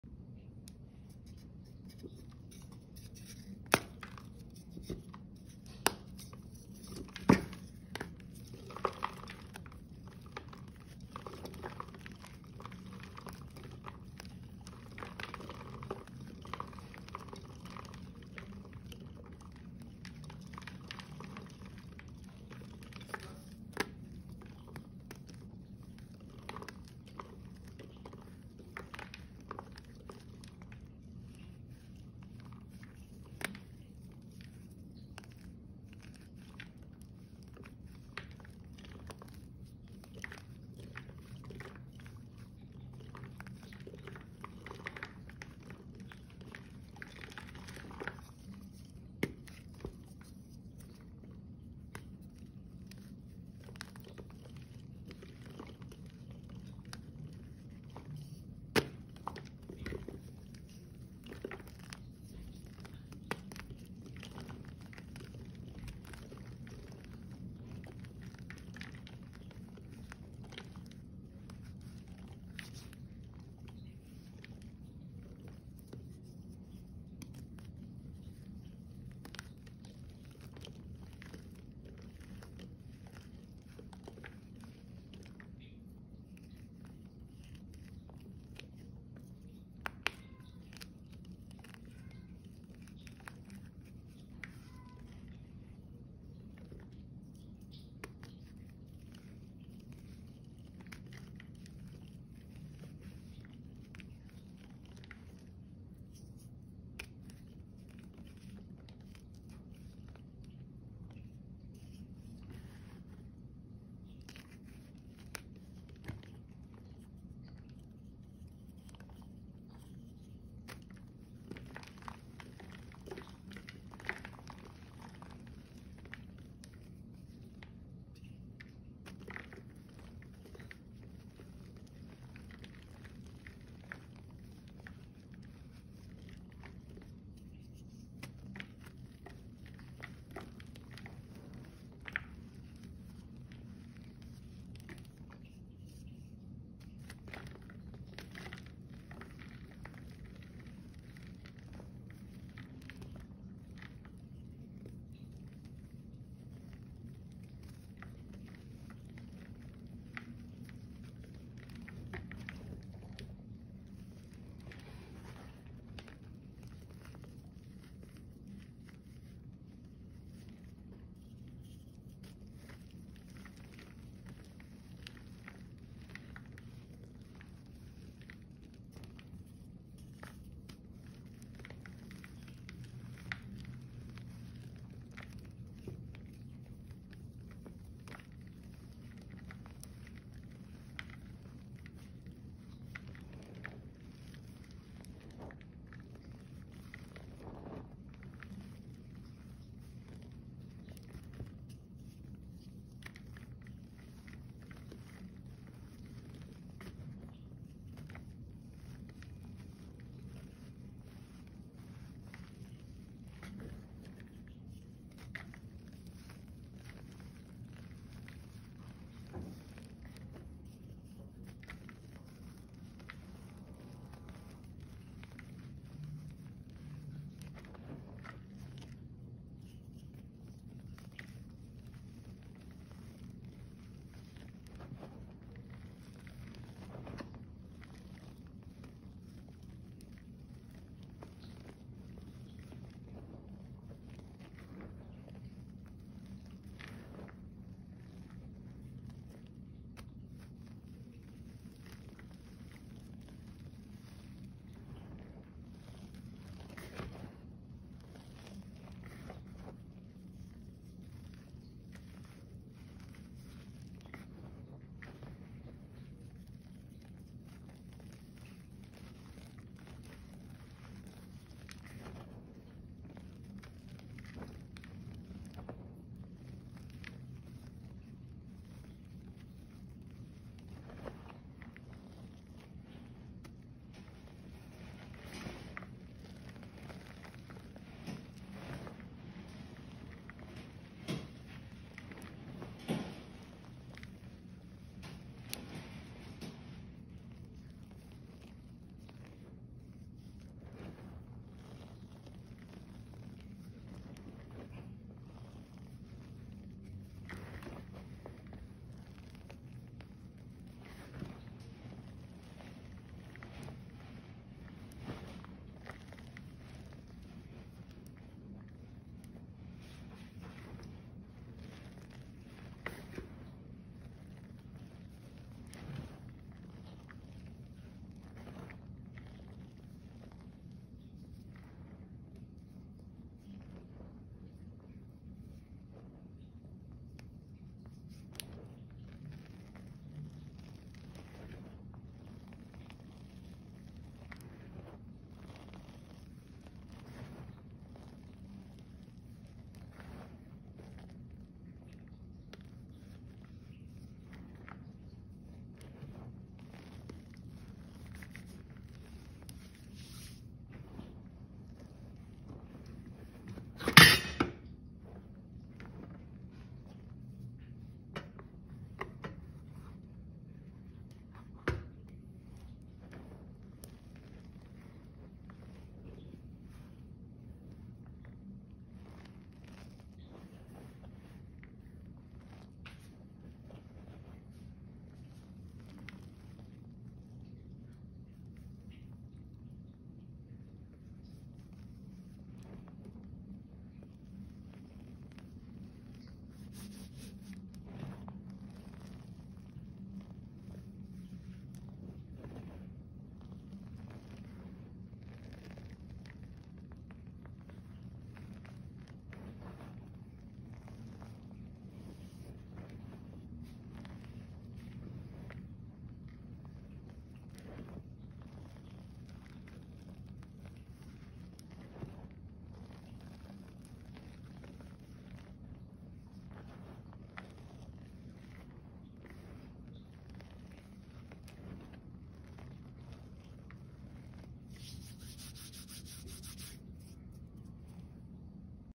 GREEN SATURDAY DAY ASMR
This was funny cause my cat toward then end started adding in his own ASMR by kicking around in his litter box. I really liked this one and the colors. Like everyone says it’s interesting how colors turn out. 💚